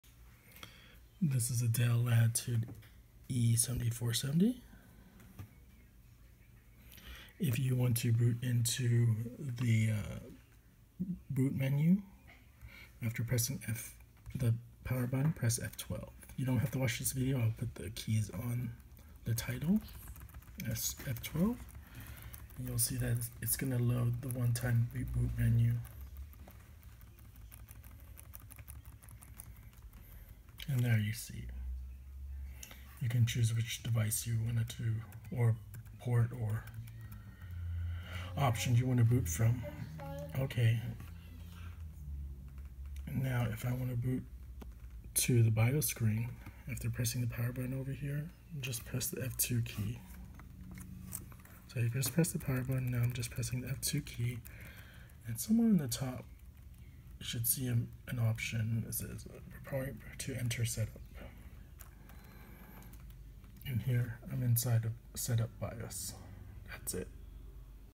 Dell Latitude E7470 / E7270 Boot Menu with F12 key. Setup Bios Menu with F2 Key. No need to watch.
I tested.. you don't have to watch the video.

If you would like to select boot sequence:
Press F12 key on bootup.

If you own a Dell E7470 Dell Laptop and would like to go into bios,..
Press F2 key on bootup.

You don't have to watch this video.. the title and description should be enough to help you.

Have a good one.  This would probably hold true for a Dell Latitude E7270